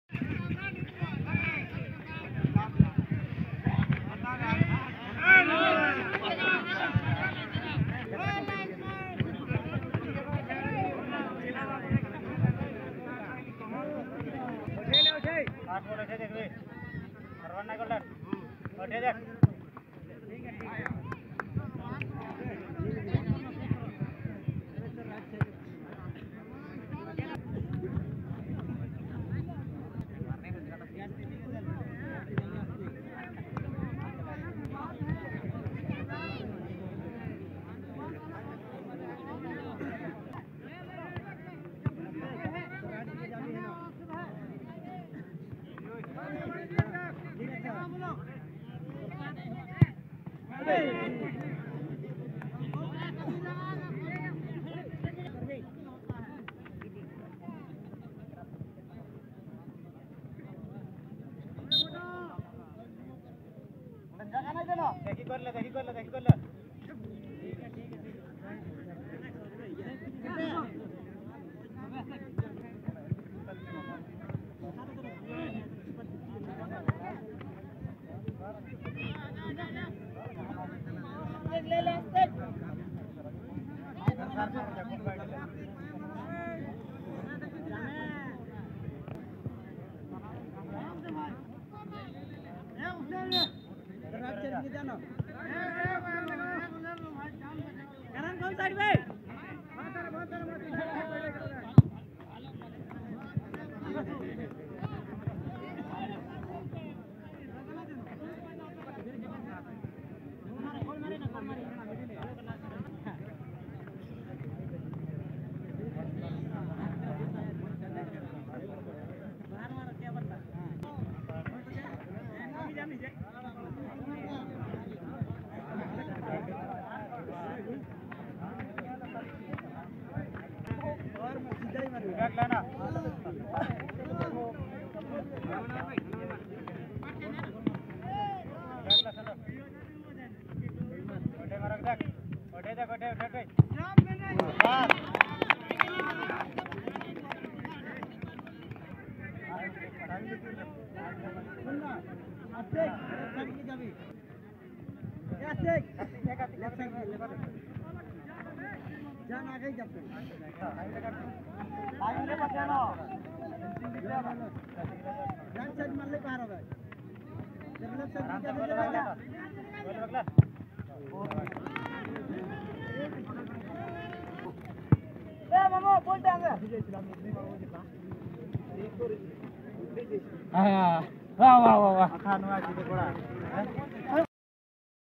Rx100 potka 🆚 D.F.C sarmanda Football tournament 2023 Semifinal pnelty 3-2 se जीत Rx100 potka
Rx100 potka 🆚 D.F.C sarmanda Football tournament 2023 Semifinal pnelty shoot 3-2 se जीत Rx100 potka